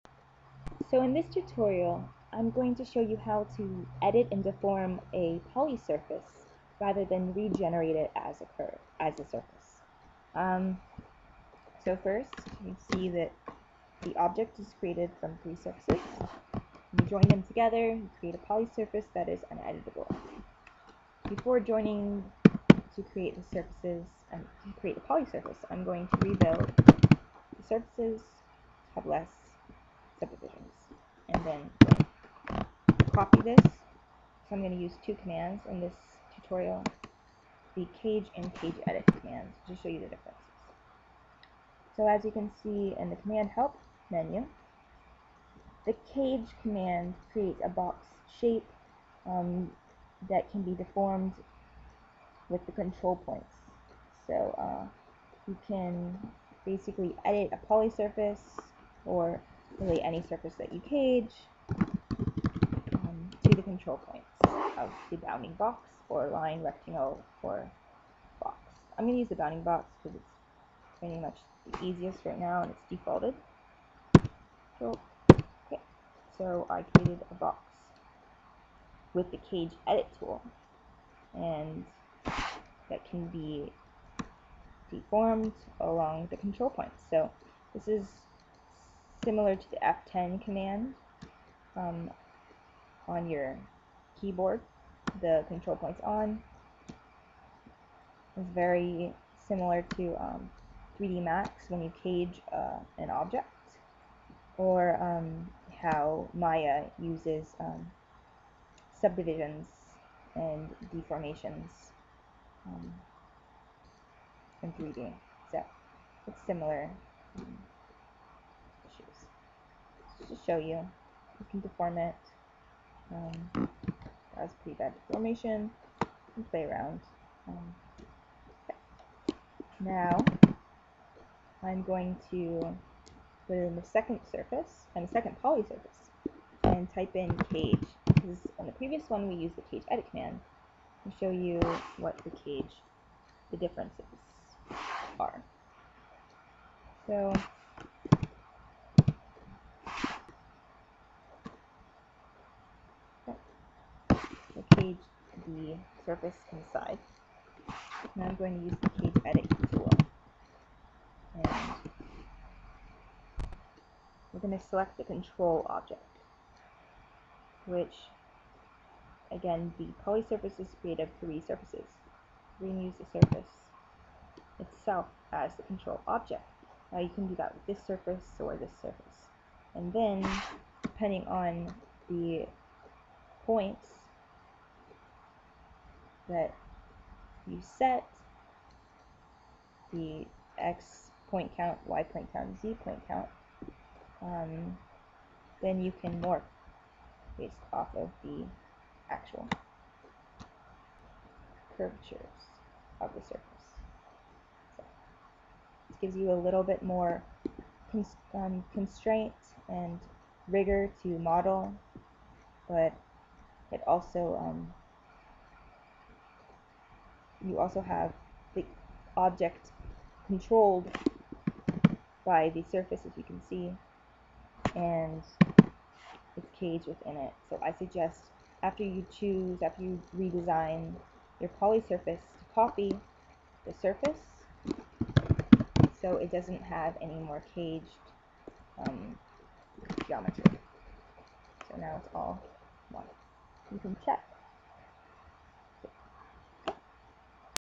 Rhino Cage / Cage Edit
Tutorial illustrates how to deform with cage / cage edit command.  Helpful to use when deforming polysurfaces. Rhino3D